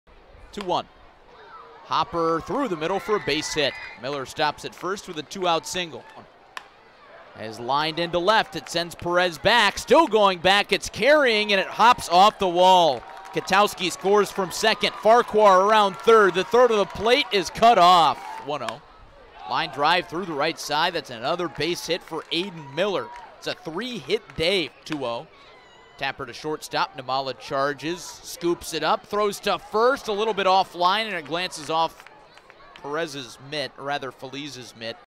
Aidan Miller collects four hits for Clearwater | MiLB Highlights
4/17/24: Phillies No. 3 prospect Aidan Miller collects four hits for Single-A Clearwater